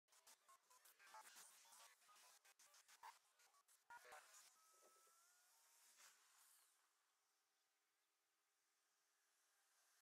100817 arroyo hondo SD 720p
Arroyo Hondo is a small unincorporated town in Taos County near Taos, New Mexico, United States. It is historically notable as the site of the killing of six to eight employees by a force of allied Native Americans at Simon Turley's mill on January 20, 1847. This took place during the Taos Revolt, a popular insurrection of New Mexicans and Native Americans against the new United States territorial regime during the Mexican-American War.